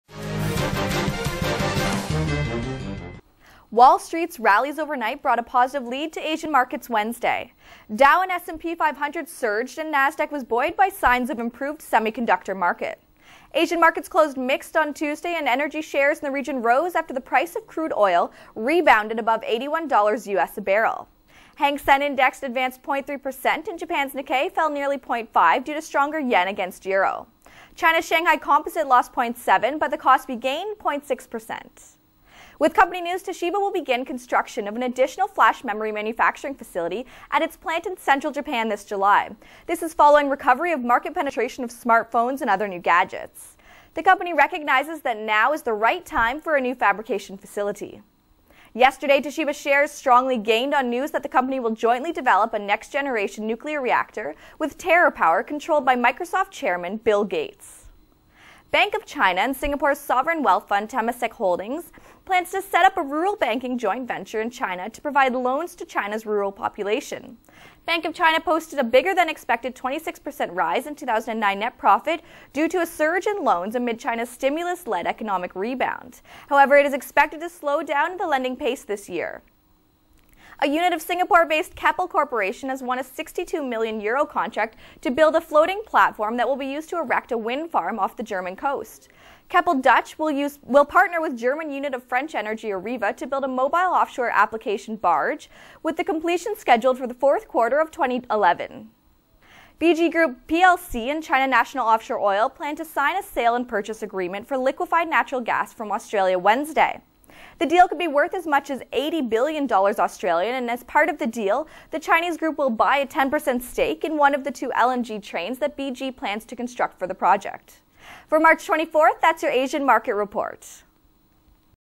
Asian Markets Overview of March 24, 2010: BG (LON:BG) and CNOOC to Sign LNG Deal
(ABN Newswire) - Wall Street's rallies overnight provided a positive lead to Asian markets on Wednesday. Dow and S&P 500 Tuesday surged to 18-month highs led by blue-chip industrial stocks. Nasdaq was also buoyed by signs of improved semiconductor market. Asian markets closed mixed on Tuesday. Energy shares in the region rose after the price of crude oil rebounded above US$81 a barrel on the New York Mercantile Exchange. Japan's Nikkei 225 fell nearly 0.5 per cent due to stronger yen against euro. China's Shanghai Composite lost 0.7 per cent. But South Korea's Kospi gained 0.6 per cent. Hong Kong's Hang Seng Index advanced 0.3 per cent.

Company News

BG Group PLC (LON:BG) and China National Offshore Oil Corp.(CNOOC), parent of Cnooc Ltd (HKG:0883), plan to sign a sale-and-purchase agreement for liquefied natural gas from Australia Wednesday, said sources. The deal could be worth as much as A$80 billion. As part of the deal, the Chinese group will buy a 10% stake in one of the two LNG trains that BG plans to construct at the project. CNOOC will also take a 5% stake in certain BG coal seam gas fields.

Toshiba Corp. (TYO:6502) said it will begin construction of an additional flash memory manufacturing facility at its plant in central Japan in July, after putting the plan on hold due to the global financial crisis. Toshiba said it saw demand begins to recover with the market penetration of smartphones and other new applications, and foreseeing further market expansion in the medium- and long-term. The company now recognizes that the time is right to construct a new fabrication facility. Yesterday Toshiba shares also strongly gained on the news that the company will jointly develop a next generation nuclear reactor with TerraPower, which is controlled by Microsoft Chairman Bill Gates.

Bank of China Ltd. (HKG:3988) said the Chinese lender and Singapore's sovereign-wealth fund Temasek Holdings plan to set up a rural banking joint venture in China. The joint venture will provide loans to China's rural population. Bank of China Tuesday posted a bigger-than expected 26% rise in 2009 net profit due to a surge in loans amid China's stimulus-led economic rebound, but the bank is expected to slow down the pace of lending this year.

A unit of Singapore based Keppel Corp (SIN:BN4) has won a 62 million euro contract to build a floating platform that will be used to erect a wind farm off the German coast. Keppel's Dutch unit will partner with the German unit of French energy company Areva (EPA:CEI) to build the Mobile Offshore Application Barge, with completion scheduled for the fourth quarter of 2011.